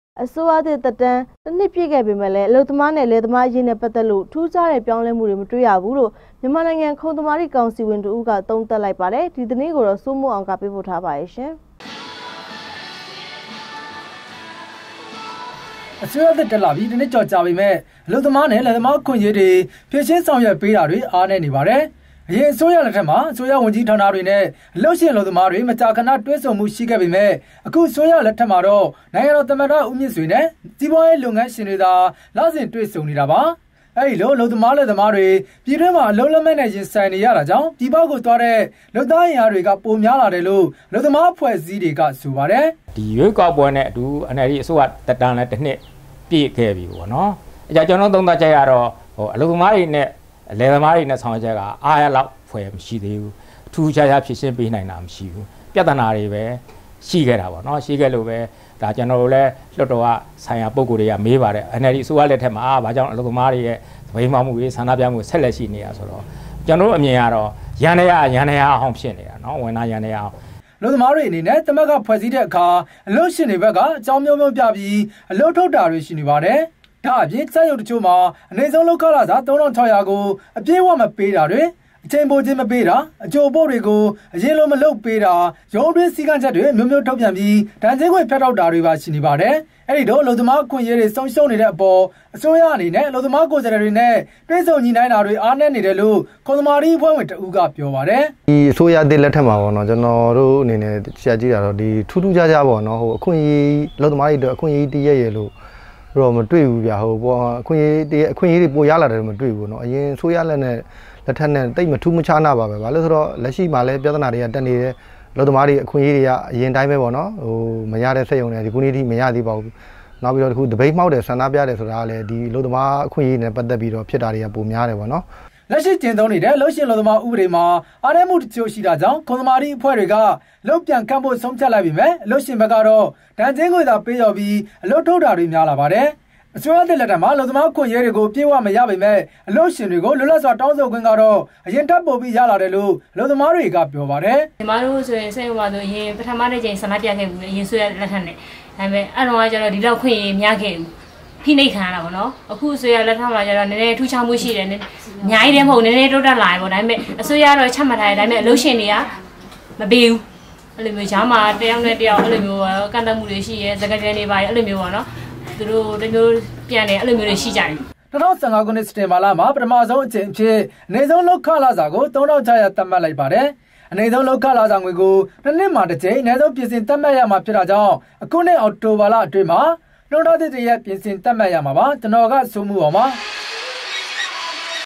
DVB TV - အစိုးရသစ္သက္တမ္း(၁)ႏွစ္ျပည့္ေပမယ့္ အလုပ္သမားနဲ႔ လယ္သမားအေရး ထူးျခားမႈမရွိ
DVB TV - အစိုးရသစ္သက္တမ္း(၁)ႏွစ္ျပည့္ေပမယ့္ အလုပ္သမားနဲ႔လယ္သမား အေရးနဲ႔ပတ္သက္လို႔ ထူးျခားတဲ့ေျပာင္းလဲမႈမေတြ႔ရဘူးလို႔ ျမန္မာႏိုင္ငံခံုသမာဓိေကာင္စီ၀င္တစ္ဦးက သံုးသပ္လိုက္ပါတယ္။ဒီသတင္းကို စိုးမိုးေအာင္ကေပးပို႔ထားပါတယ္။